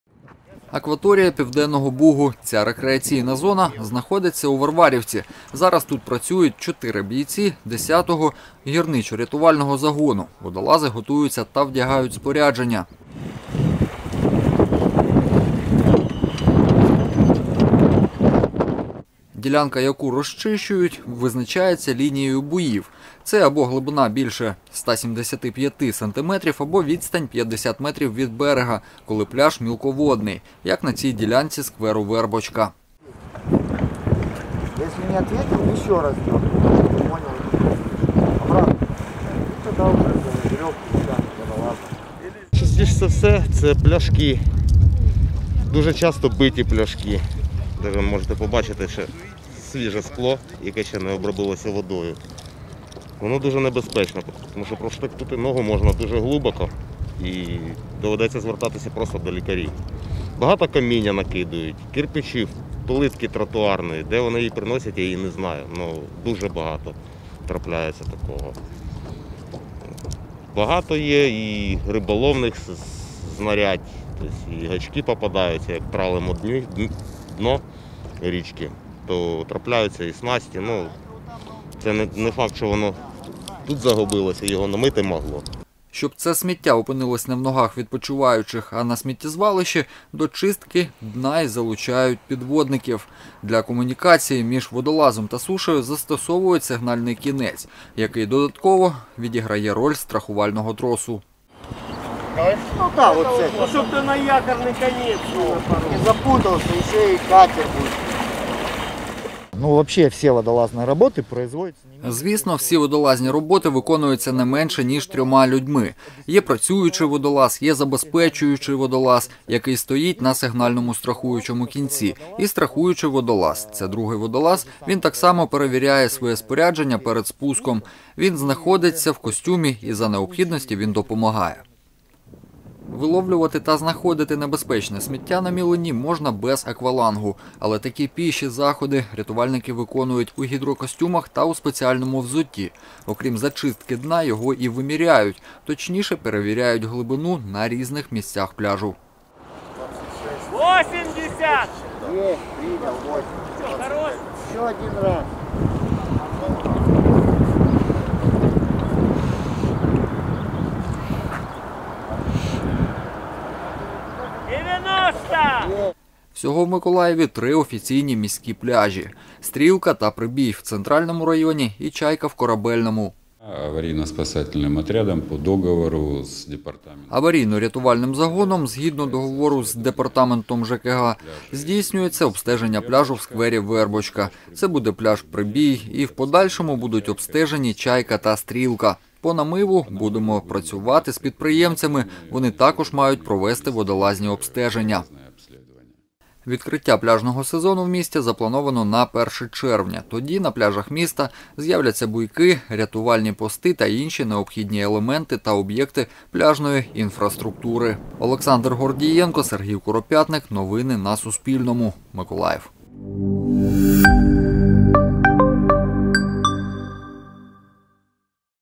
Миколаївські пляжі та зони відпочинку готують до літнього сезону
У Миколаєві розпочали підготовку міських пляжів та зон відпочинку до літнього сезону. Водолази обстежують дно пляжної ділянки скверу «Вербочка» та замірюють глибину води в цій рекреаційній зоні.
Автор - Олександр Гордієнко, оператор - Сергій Куропятник.